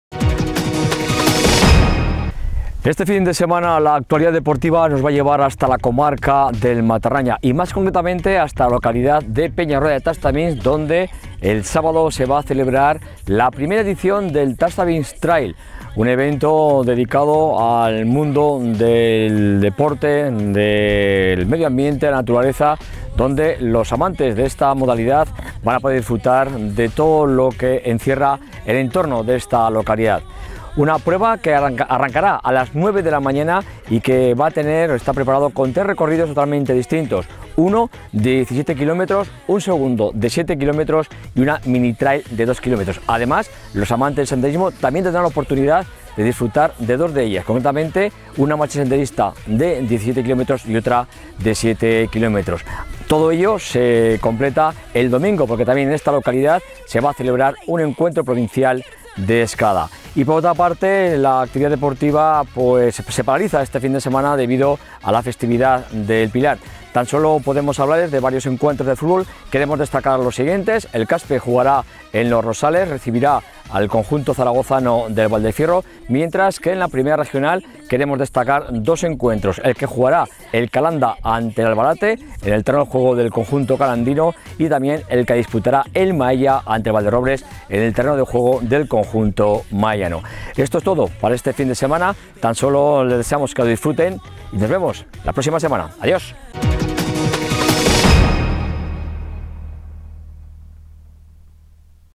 Los deportes fin de semana 14 y 15 octubre de 2017
Agenda deportiva para el fin de semana que nos presenta ,Javier Vílchez. La festividad del Pilar hace parar las competiciones aun así tenemos varias actividades para este fin de semana.